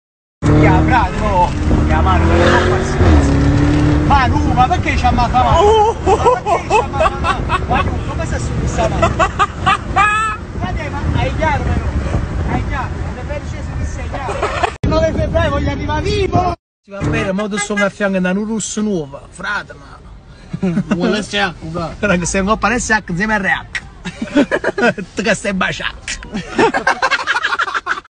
Rocco Hunt in auto con Geolier
Geolier che corre in auto con Rocco hunt